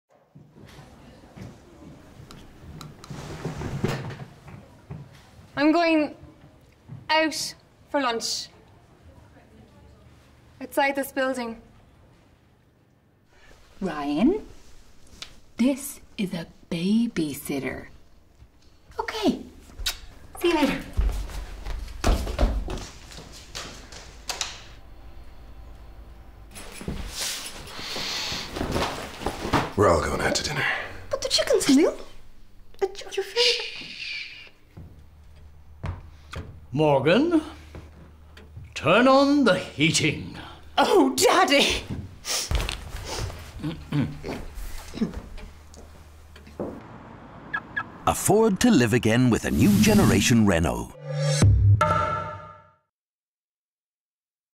Renault - 'Afford to Live Again' - TV commercial
Commercial I directed for Publicis / Renault

Director: Lorcan Finnegan
Agency: Publicis Dublin
Senior Copywriter: Des Creedon
Senior Art Director: Ger Roe
Agency Producer: Niamh Skelly
Producer: Helen Hayden
Executive Producer: Michael Duffy